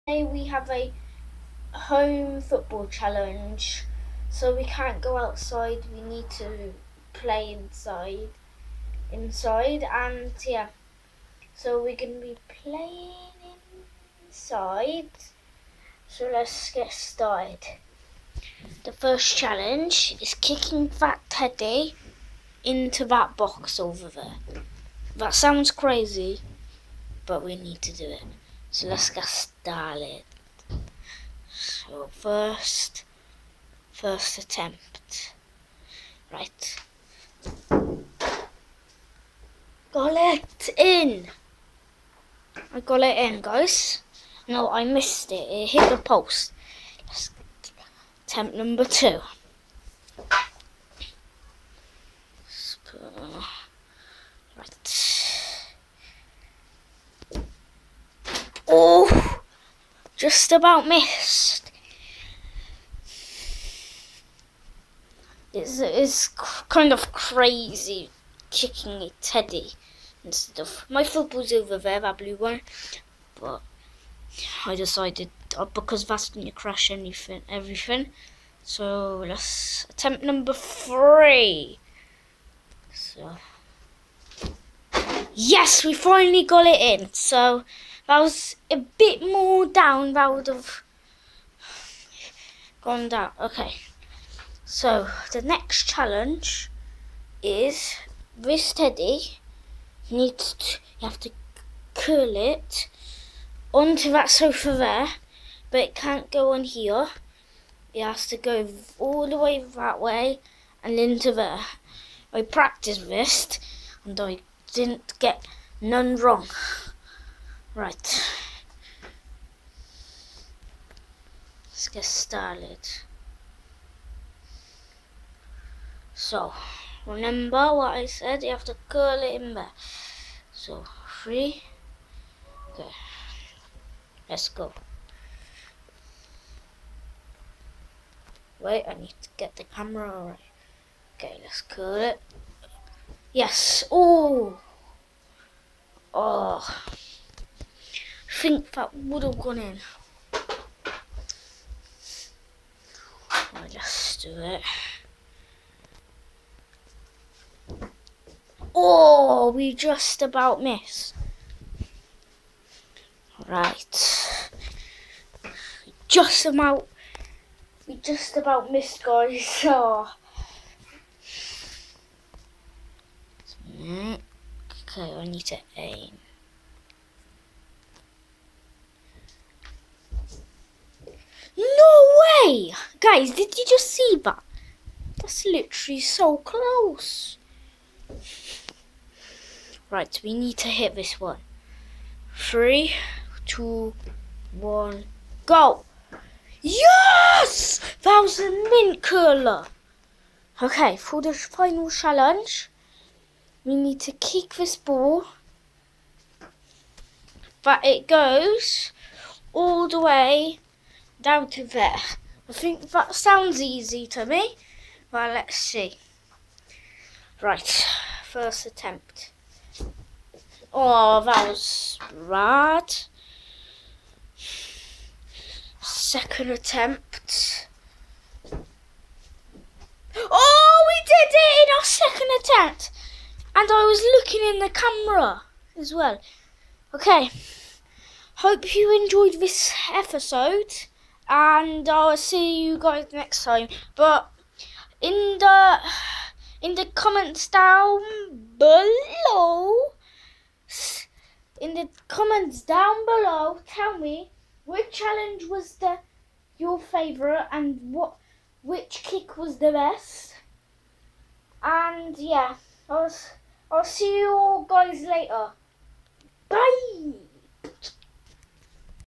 House hold football challenges with a teddy 🐻
Very crazy kicking a bear 🐻 around